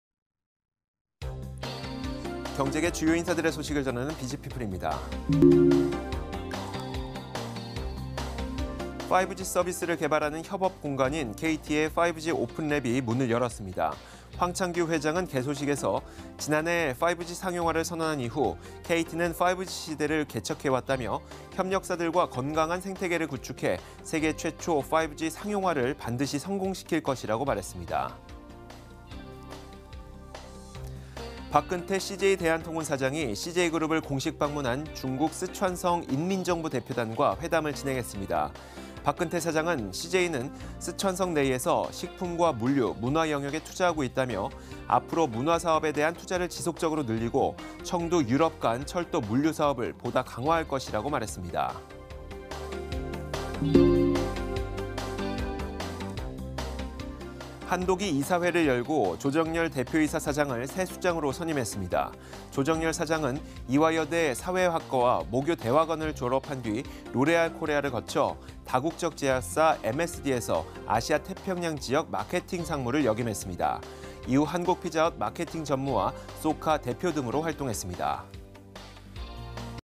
[비즈피플] 황창규 KT 회장 "세계 최초 5G 상용화 성공시킬 것" 외 2건 / 머니투데이방송 (뉴스)
09/04 MTN 투데이

경제계 주요 인사들의 소식을 전하는 비즈피플입니다.

1. 황창규 KT 회장 "세계 최초 5G 상용화 성공시킬 것"
2. 박근태 CJ 대한통운 사장, 中 쓰촨성 대표단과 회담
3. 한독, 조정열 신임 대표이사 사장 선임